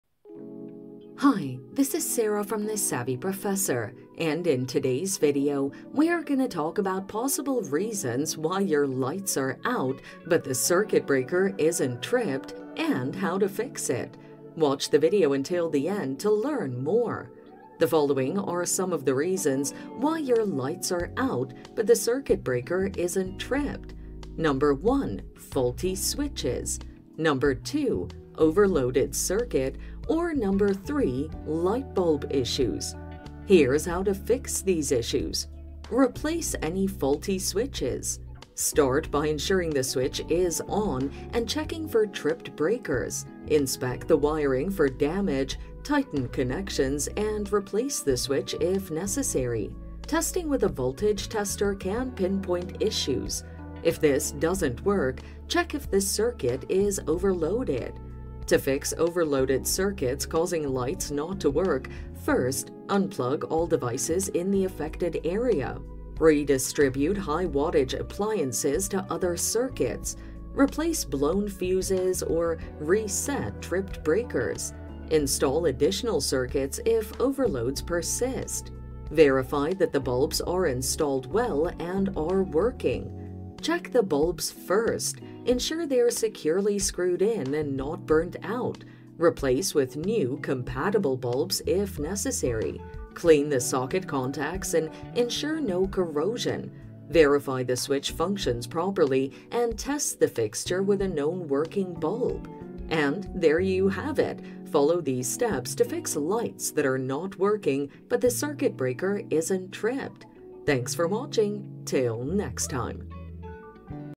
How To Fix Lights Not Working But The Fuse Is Not Tripped (Possible Causes And Simple Solutions!)
How To Fix Lights Not Working But The Fuse Is Not Tripped (Possible Causes And Simple Solutions!). In this video tutorial I will show you how to fix lights not working but the fuse is not tripped.

The following are some of the reasons why your lights are out but the circuit breaker is not tripped:
1. Faulty switches
2. Overloaded circuit
3. Light bulb issues

Here’s how to fix these issues;
Replace any faulty switches
Start by ensuring the switch is on and checking for tripped breakers. Inspect wiring for damage, tighten connections, and replace the switch if necessary. Testing with a voltage tester can pinpoint issues.

If this doesn’t work,
Check if the circuit is overloaded
To fix overloaded circuits causing lights not to work, first, unplug all devices in the affected area. Redistribute high-wattage appliances to other circuits. Replace blown fuses or reset tripped breakers. Install additional circuits if overloads persist.

Verify that the bulbs are installed well and are working
Check the bulbs first. Ensure they're securely screwed in and not burnt out. Replace with new, compatible bulbs if necessary. Clean the socket contacts and ensure no corrosion. Verify the switch functions properly, and test the fixture with a known working bulb.